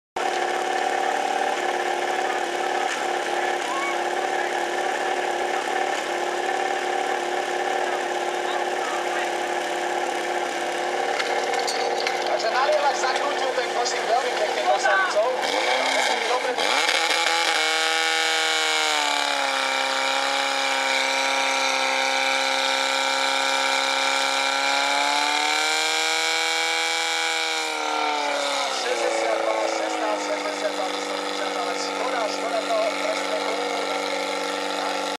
DHZ Bartošova Lehôtka muži XII. ročník súťaže o Putovný pohár starostu obce Socovce 5.9.2015
XII. ročník súťaže o Putovný pohár starostu obce Socovce 5.9.2015
DHZ Bartošova Lehôtka muži
4.miesto
čas: 16,62